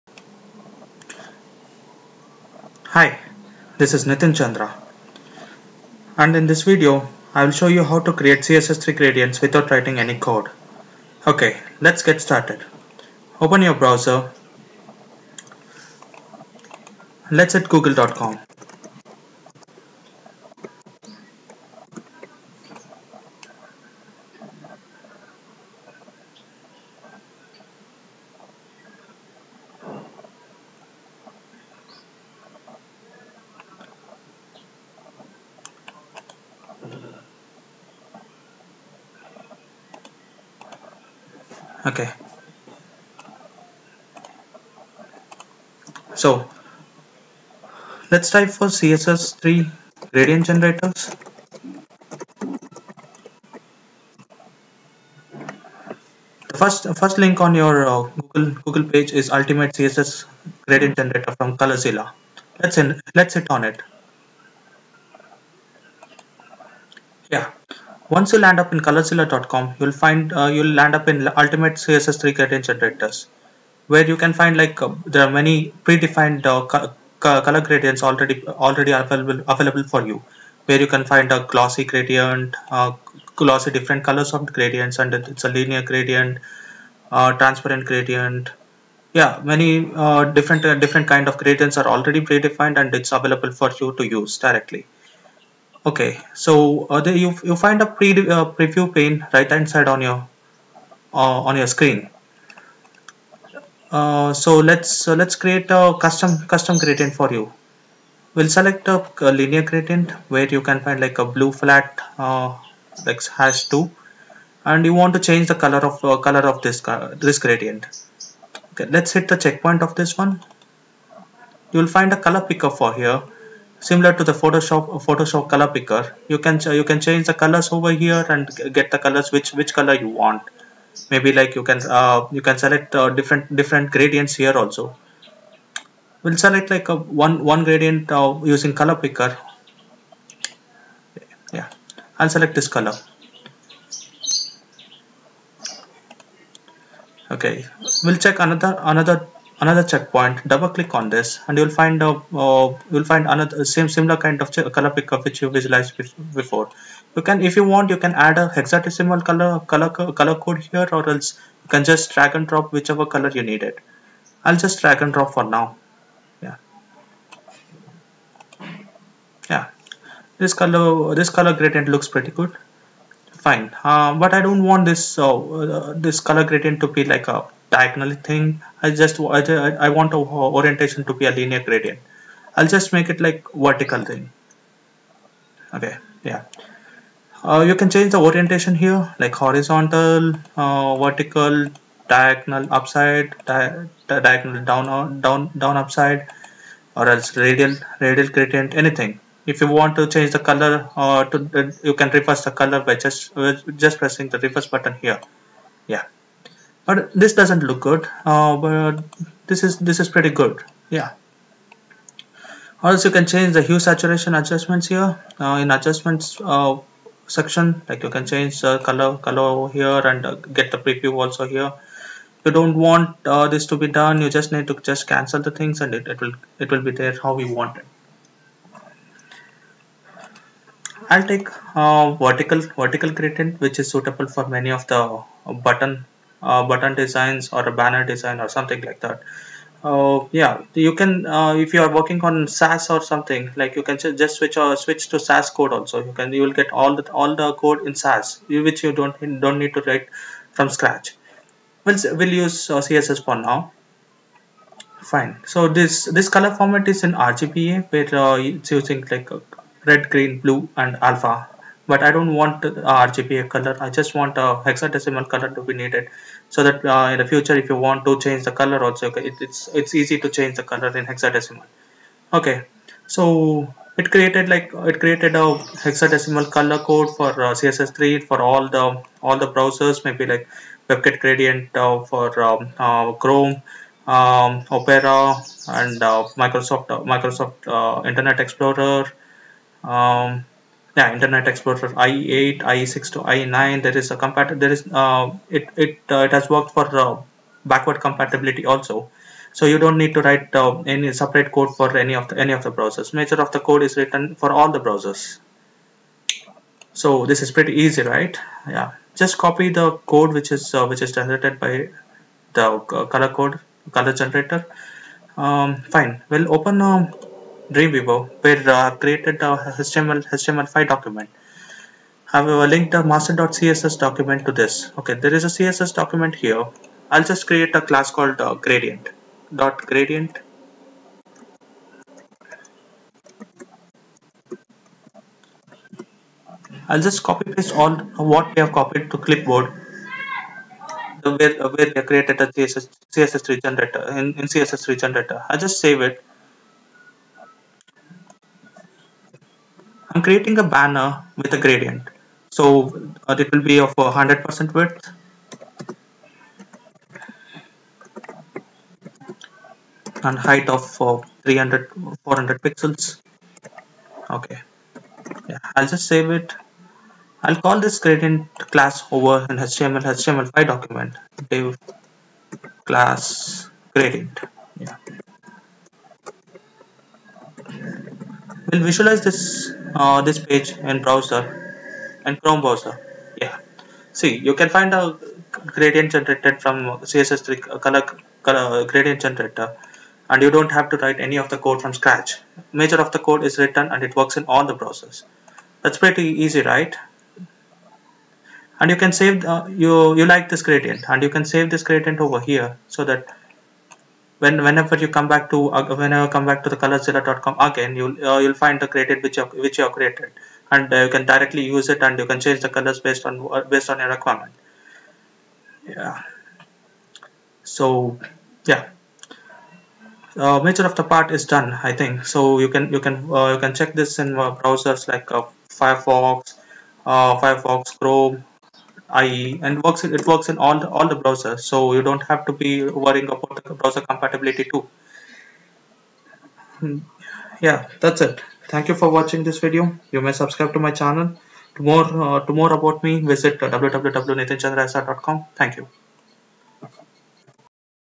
Photoshop-like CSS3 Gradients
Skill Mode: Beginner & Intermediate
Many of web designers are now in use with CSS3 elements like border radius, gradients, transitions. But many of us face problems in using CSS3 properties mentioned above like - browser compatibility issue,complex CSS3 codes . In this video I will explain you how to create Photoshop like CSS3 gradients for web without any hassle and with less time. To know more about me